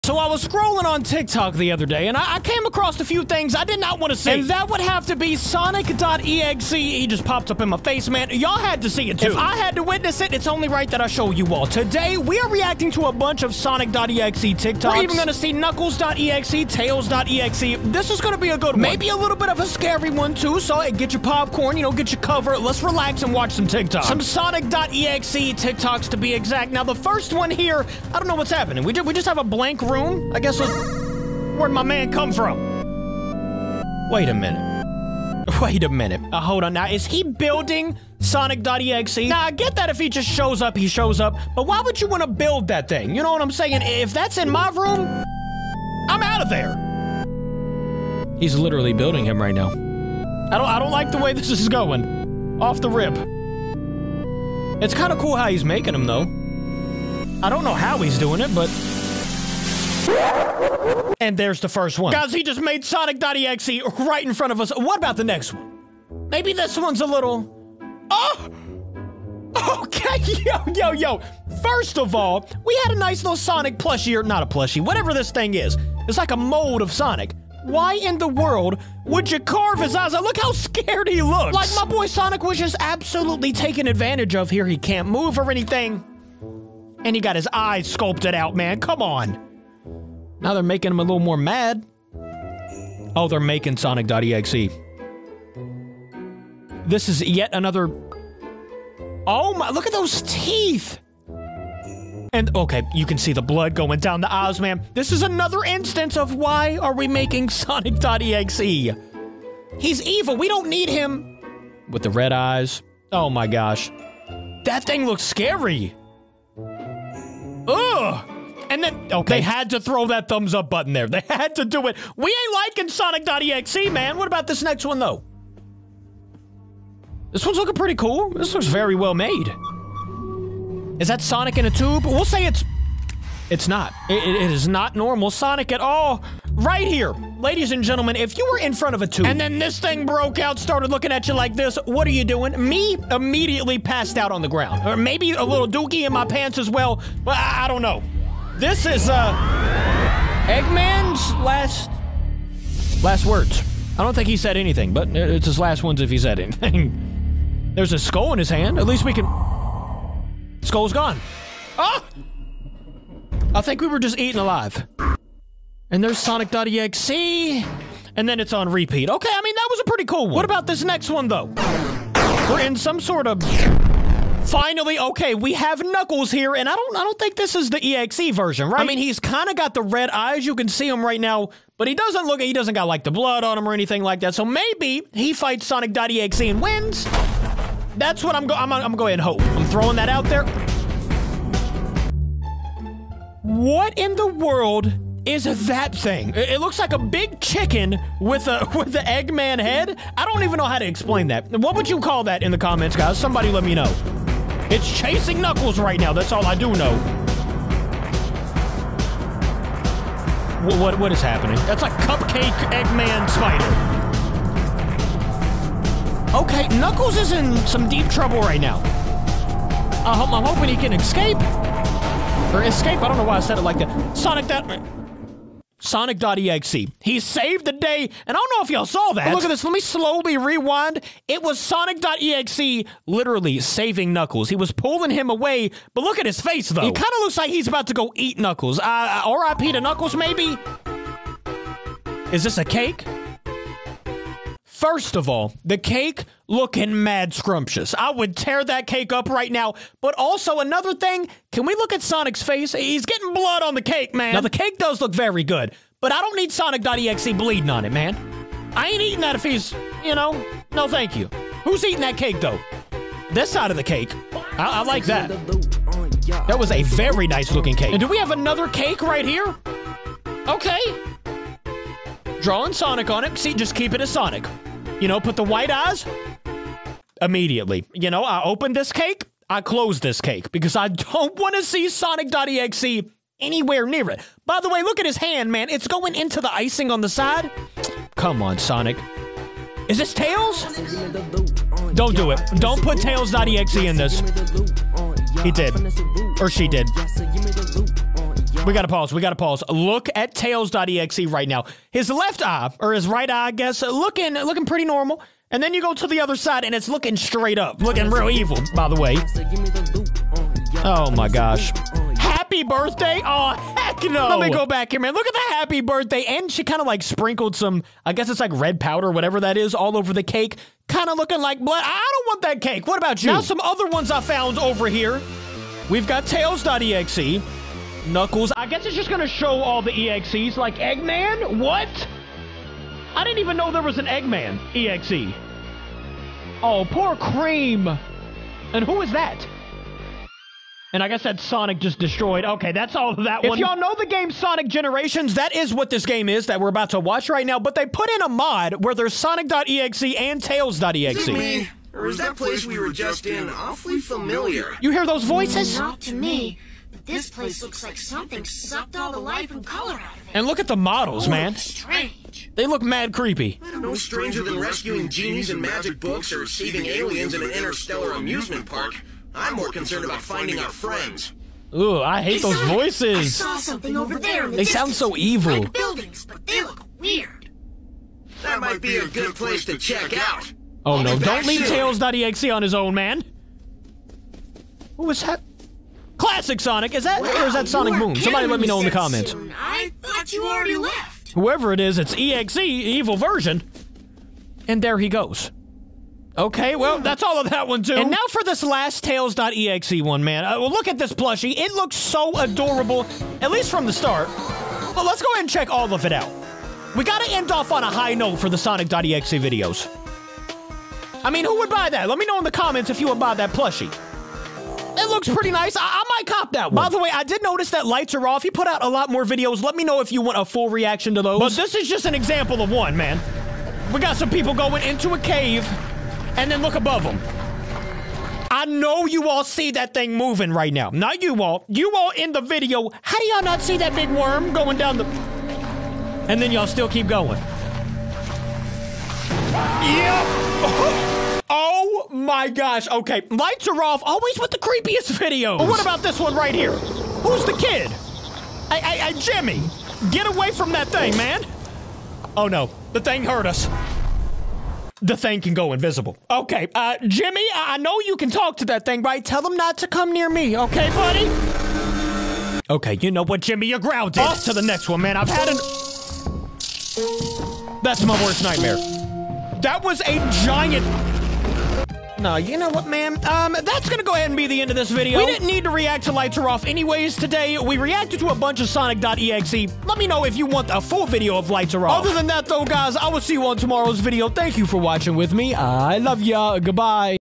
Reacting to CURSED SONIC.EXE Tik Tok Videos!
Finding CURSED Sonic.EXE videos on Tik Tok and reacting to them!!